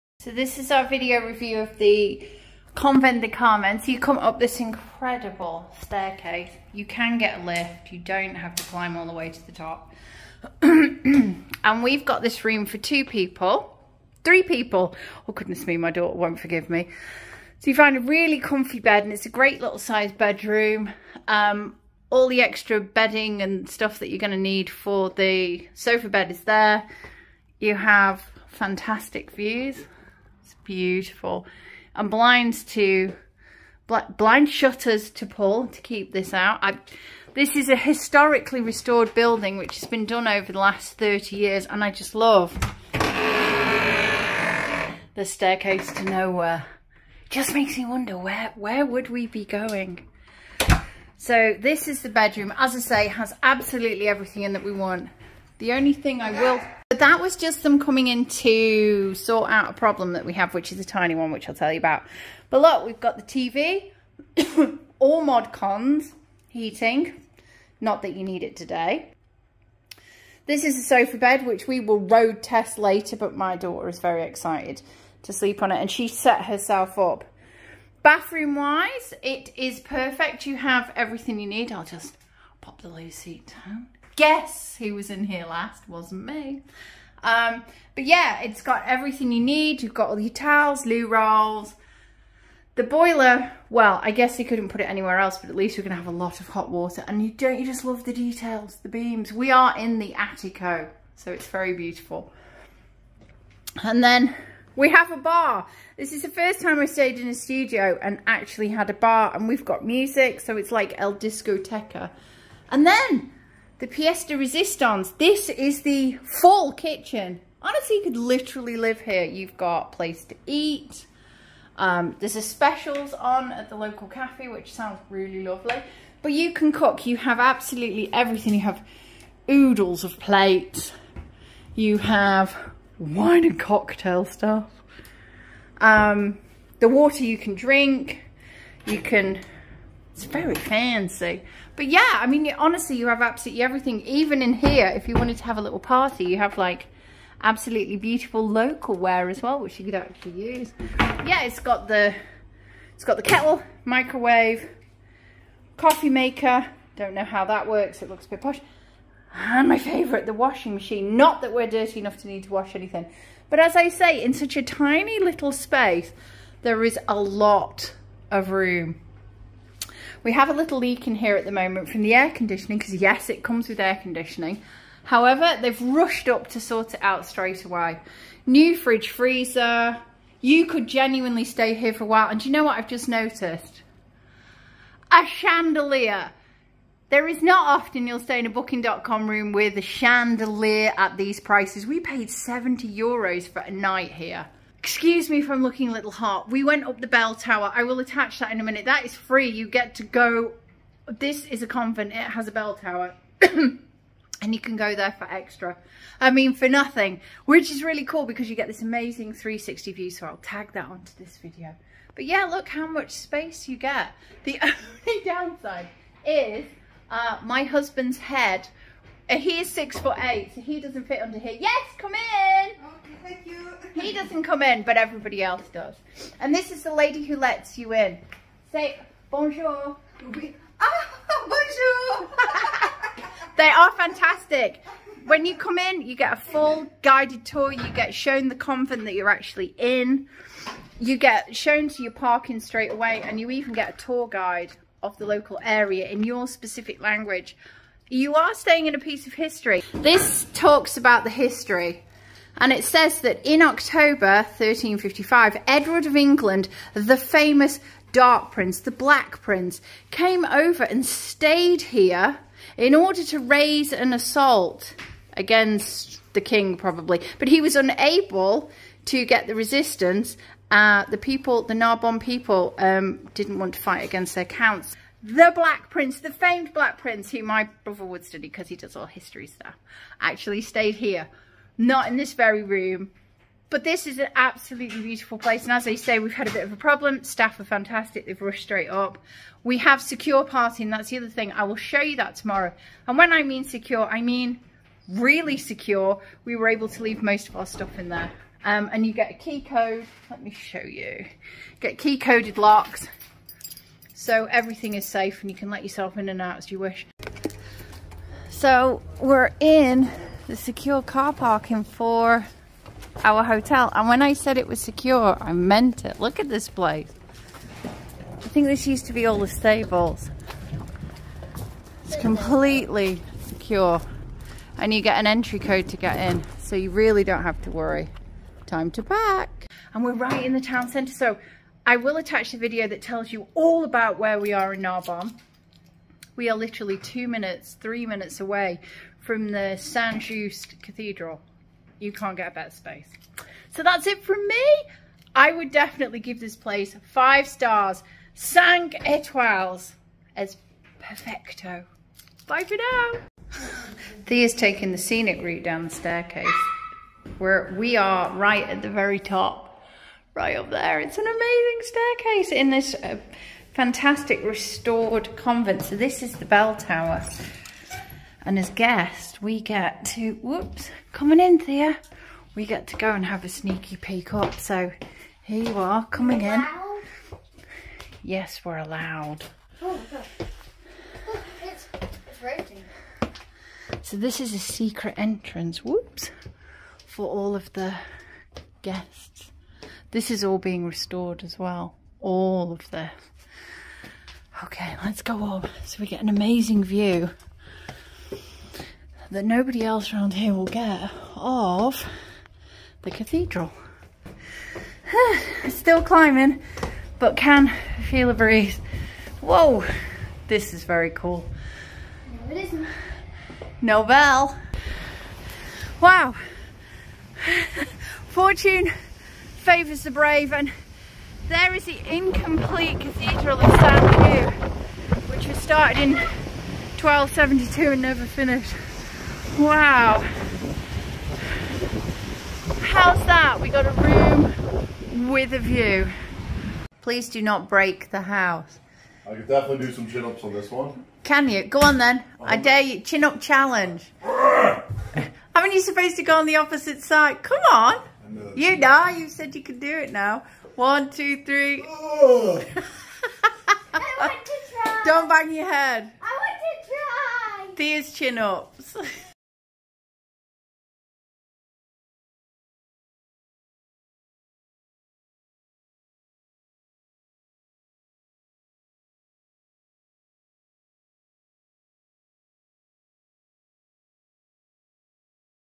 Where To Stay Narbonne - Studio De Convent, Couvent Des Carmes Review
On our recent European Road Trip we travelled through the beautiful city of Narbonne, France a place we would highly recommend to go to. We stayed in the fantastically located Studio De Convent in central Narbonne which was only a small walk away from all the tourist attractions, restaurants and shops. There is also great secure parking available for your car.